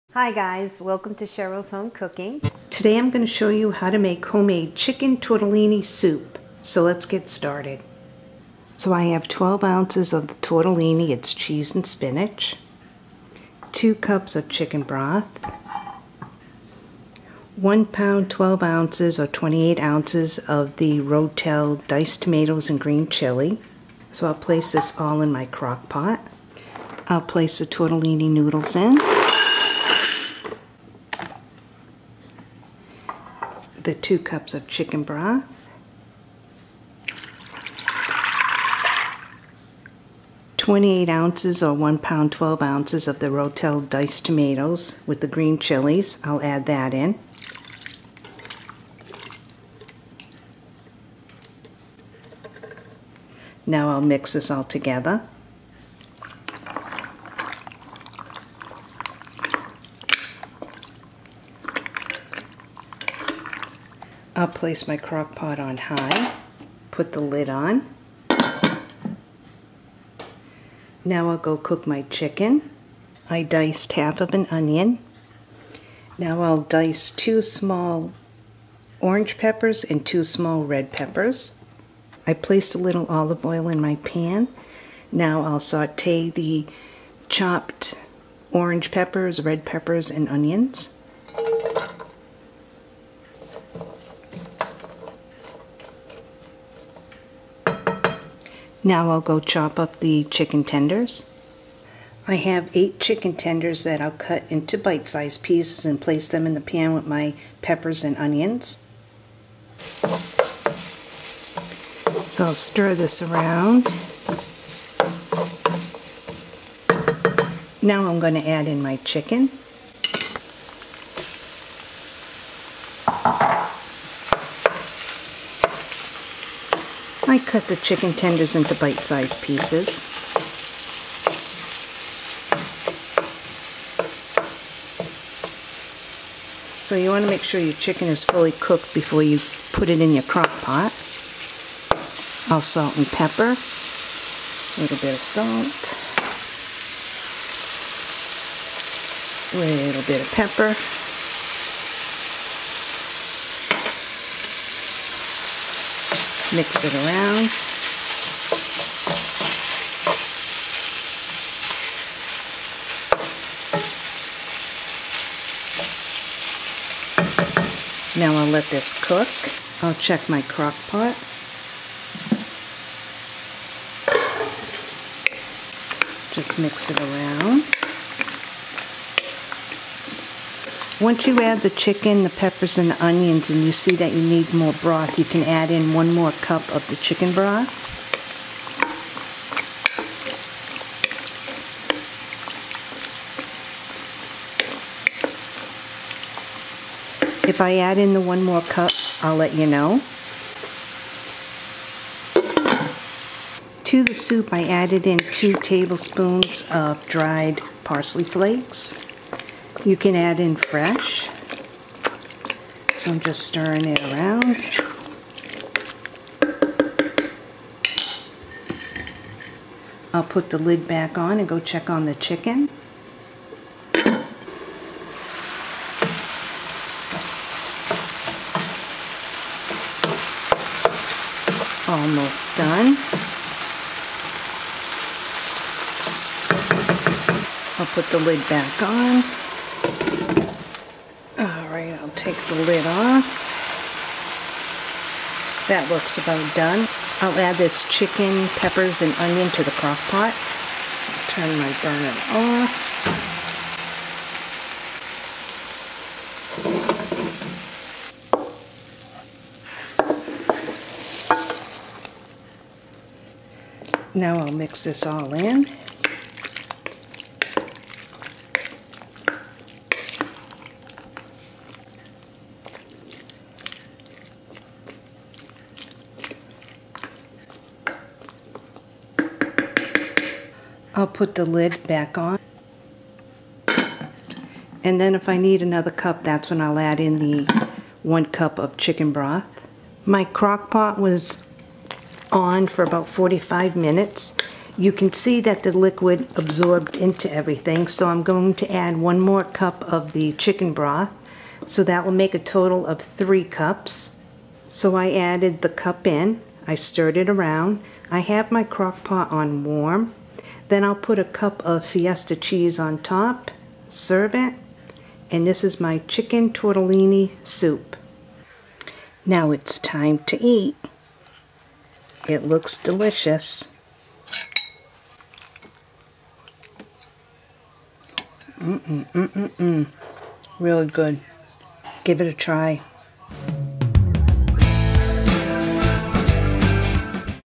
QUICK AND EASY SOUP/ CROCK POT/ HOME MADE/CHICKEN TORTELLINI SOUP/EPISODE 884/CHERYLS HOME COOKING
In this video I show you how to make home made "Tortellini Soup" in the crock pot.

INGREDIENTS:

3-Cups of chicken Broth
28 oz. can of Roe- tel ( Diced Tomatoes and green chilies)
12 oz. Tortellini Noodles
1- Cup of "Fiesta Cheese"( Monterey Jack & Cheddar Blend)
4- Mini Peppers ( any color will do)
1/2 Onion
8- Chicken Tenders... OR ( 1  Boneless Chicken Breast)
2- Tbs.-Parsely Flakes... (fresh or dry)
Salt/Pepper
Olive Oil

DIRECTIONS:

Place your crock pot on high.
Pour the bag of Noodles into your crock pot...
Pour 2 cups of Chicken Broth into the crock pot...
Pour your can of Ro Tel into the crock pot.
Give this a stir.
While this is in your crock pot on high cooking.
Chop your 1/2 onion and your 4 peppers.
Place a little  Olive oil in your frying pan...
Saute the onions and peppers.
Chop your Chicken tenders into bite size, and place into your frying pan with the onions and peppers.
Salt and pepper the chicken and mix.
Cook the chicken until fully cooked.
Add 2- Tbs of Parsley...(dry or fresh)
Mix.
Place the Chicken,Onions,and Peppers into your crock pot.
Mix well.
Add the last cup of Chicken broth in and stir.
Cook for about 45 minutes or until the noodles are tender.
Place your crock pot on warm.
Add the 1 cup of Fiesta Cheese...
Mix...Let the cheese melt a little...( about 10 minutes or less)
Serve the Tortellini in a bowl and ENJOY!